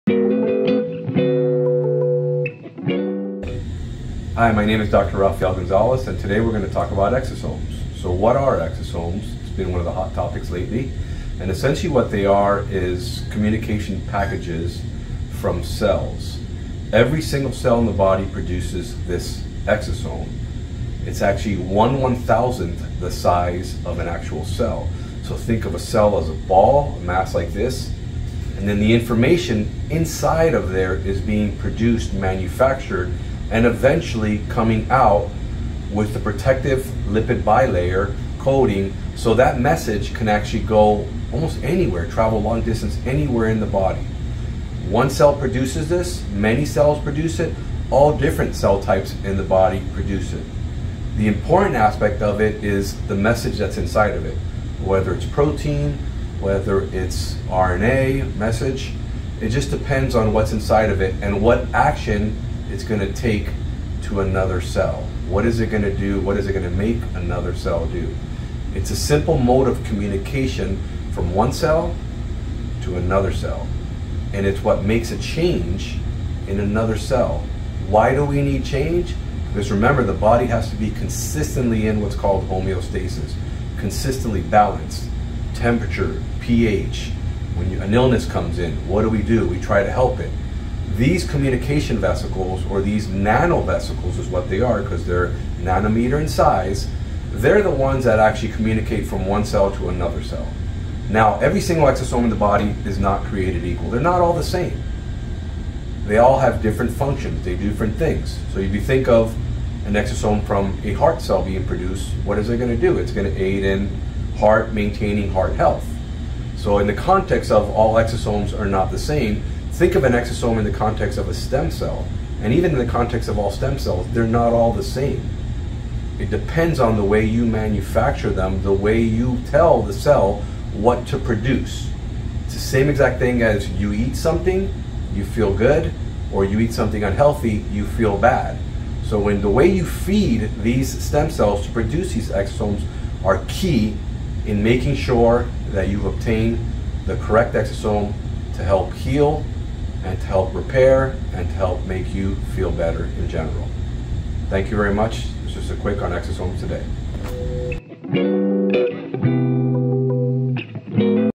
Dr Rafael Gonzalez - What are exosomes?
What are exosomes?

The discovery of these incredible nanoparticles has created a buzz in all aspects of medicine. Dr. Rafael Gonzalez discusses what they are and their extraordinary potential.